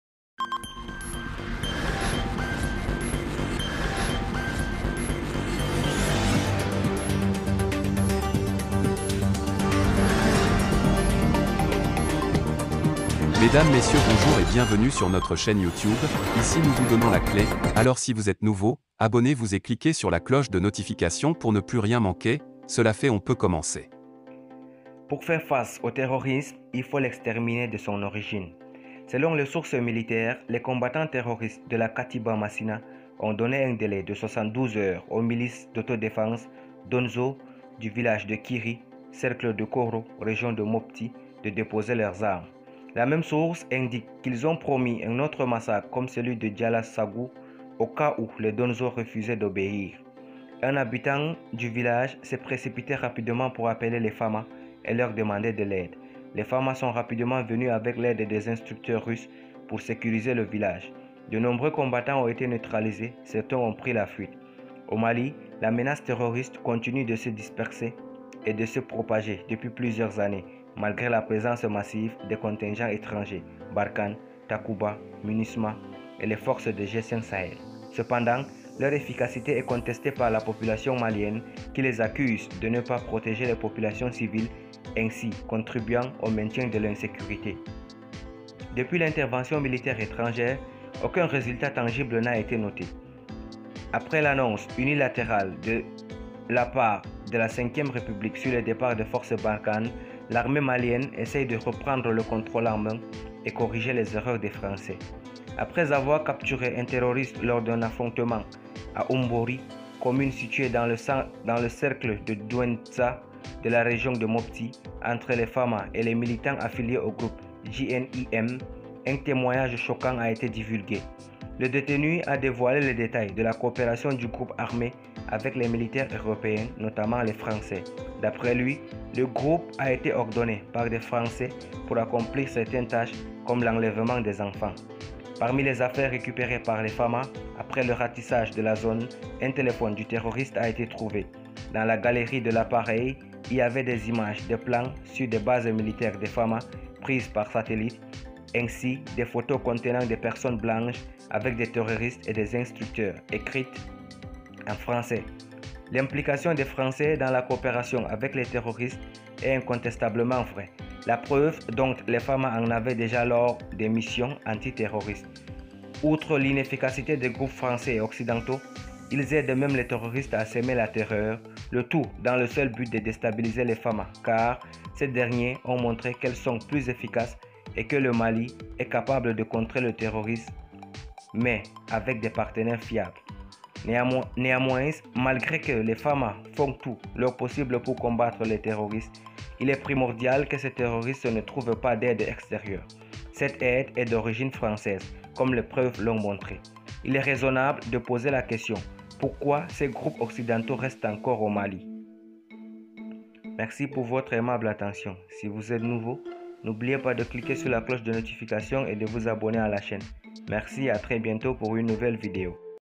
Mali : voici comment la FAMA faire une montée en puissance sans...
l'armée malienne monte en puissance avec les nouvelles armes qui permet une offensive contre les terroristes